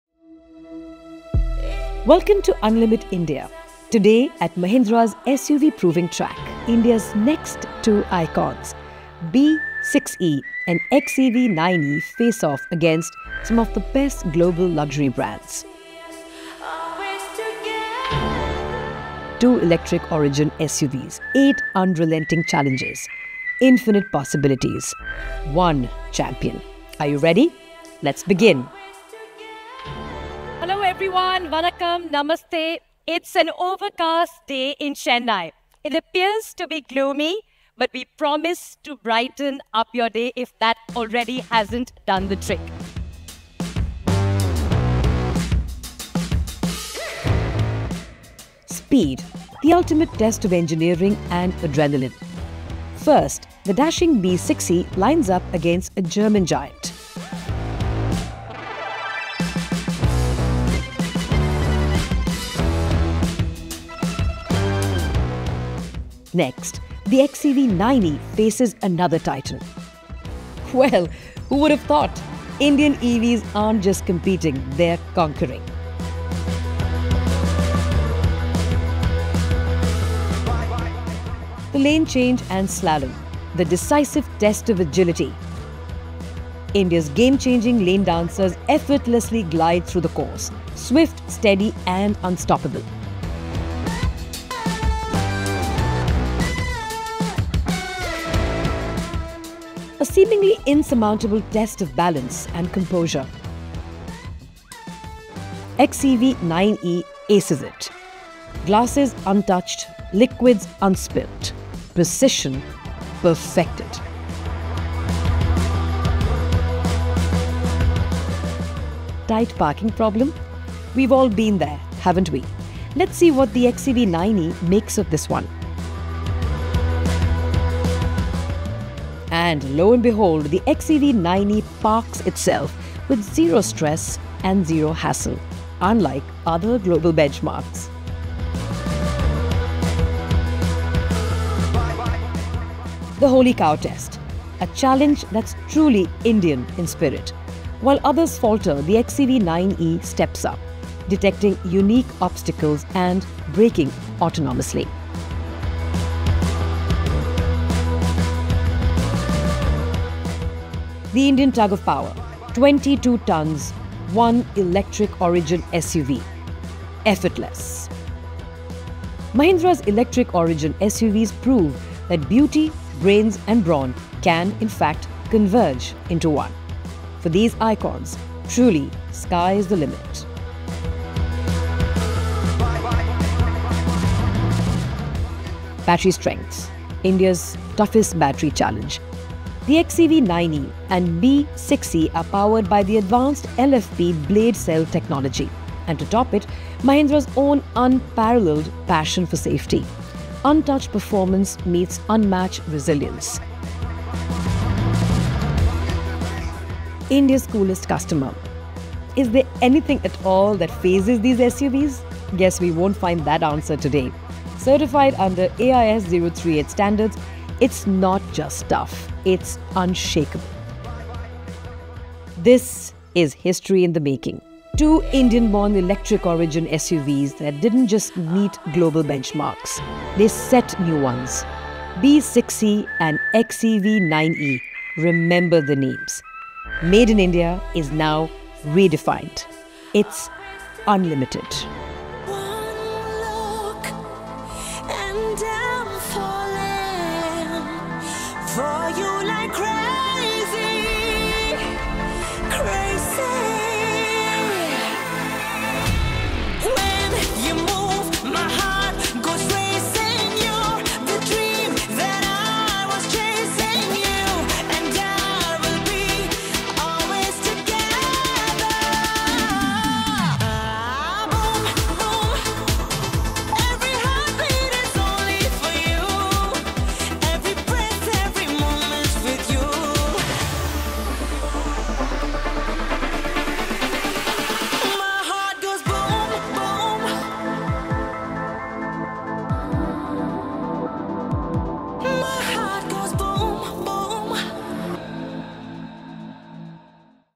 Unlimit India | Day 2 Highlights | Mahindra's Electric Origin SUVs take on 8 epic challenges
At the Mahindra SUV Proving Track, India’s next icons — BE 6 and XEV 9e — took on 8 unforgiving challenges and emerged victorious, making the nation proud with their unparalleled performance.

From agility to power, from precision to resilience, these electric-origin SUVs didn’t just meet global benchmarks — they surpassed them, setting new standards in automotive excellence with cutting-edge technology.

Catch the action and highlights from Day 2 of Unlimit India now.